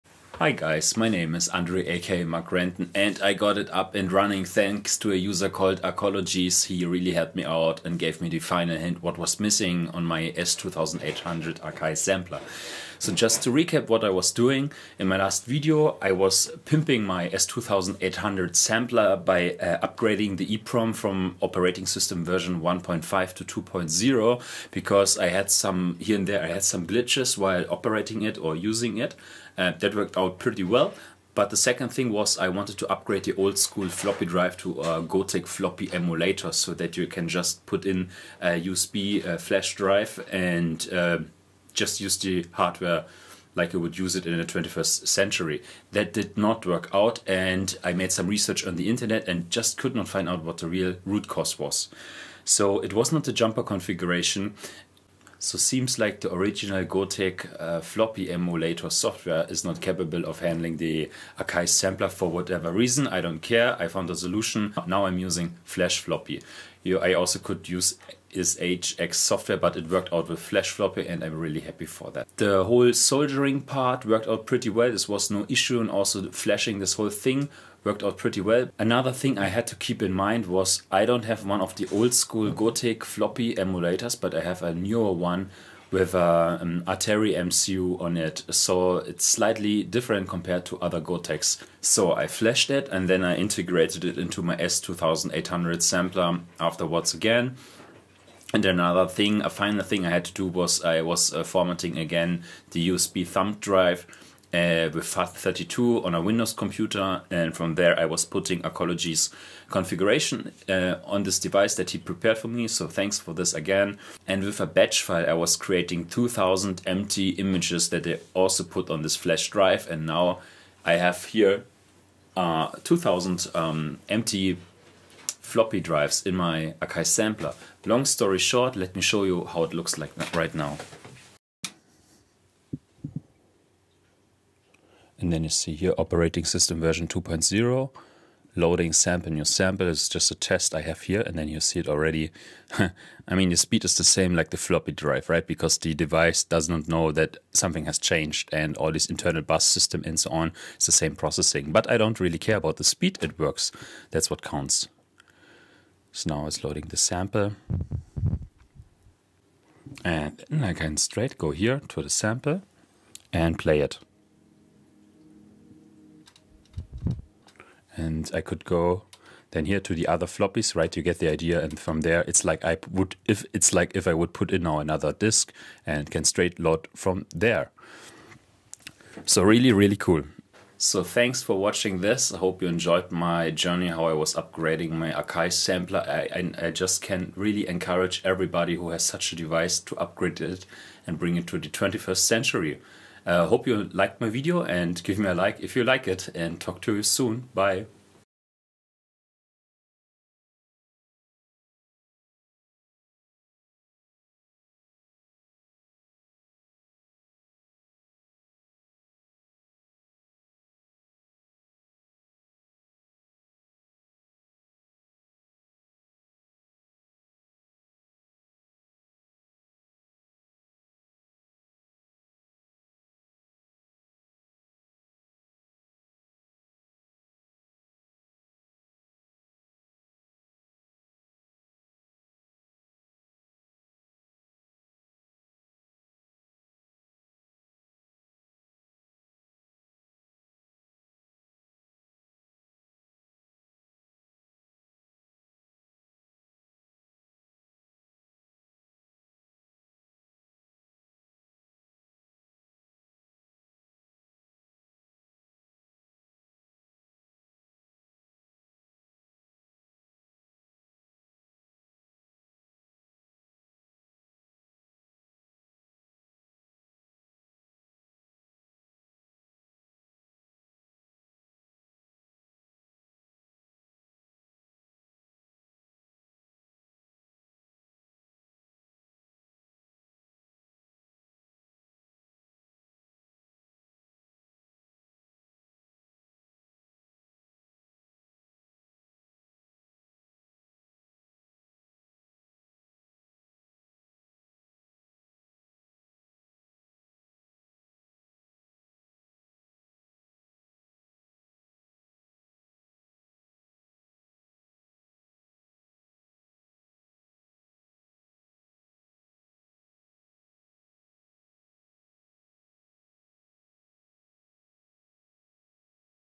Akai S2800 Gotek FlashFloppy Emulator WORKS!
YES YES YES my Akai S2800 Sampler Gotek FlashFloppy Emulator FINALLY WORKS! Thanks to user arcologies' help I finally made it work
Main issue I solved:
- The Gotek Floppy Emulator NEEDS an alternative firmware, eg HXC2001 or FlashFloppy
- My Gotek is a newer one with an Artery MCU. Hence, the flash-process differs a little bit
- Configuration depends on the device but is well documented by both projects
- I formated my flashdrive via FAT32
- Finally, the configuration + images (equal in empty floppy disks) have to be put on the flashdrive